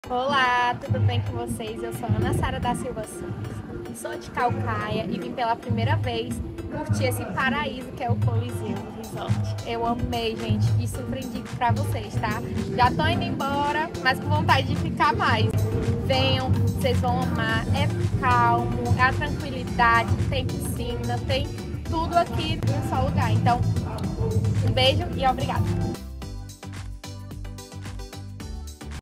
Ana Sara de Caucaia-CE Encantada com a Tranquilidade do Coliseum Beach Resort!
Ana Sara da Silva Santos, de Caucaia, Ceará, compartilha sua primeira experiência no Coliseum Beach Resort. Ela destaca a calma e a tranquilidade do local, além das maravilhosas piscinas. Já está indo embora, mas com vontade de ficar! Venha e sinta a magia deste lugar. 🌴☀️

👉 Saiba mais em nosso site: coliseumbeachresort.com.br
Não se esqueça de seguir-nos no Instagram e Facebook para mais informações e promoções exclusivas.